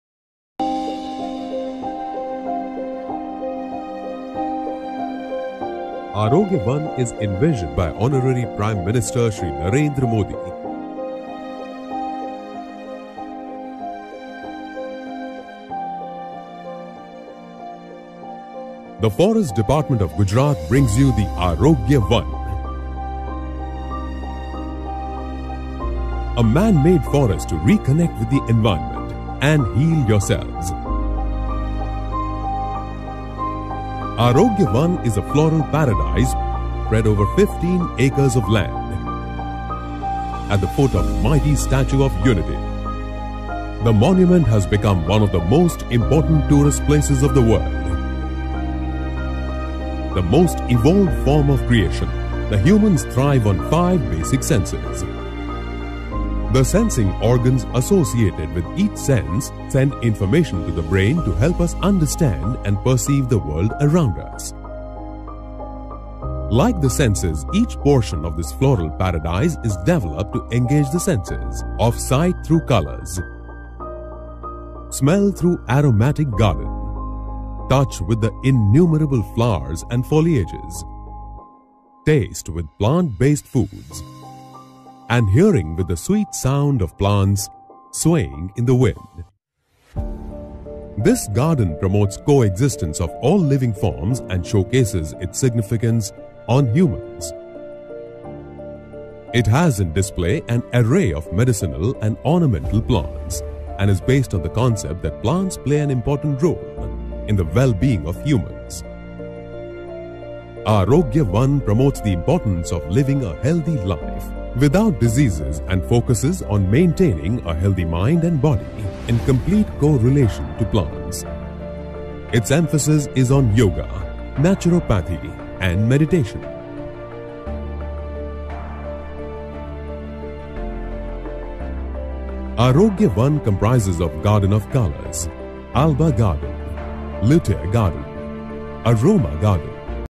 Arogya Van - Part 1 | Gujarat Forest Department
Gujarat Forest Dept brings you a unique concept of ArogyaVan A manmade forest to reconnect with Nature,spreaded over 15 acres.
This floral paradise signifies all living forms and importance of living healthy life with emphasis on yoga. Part 1/3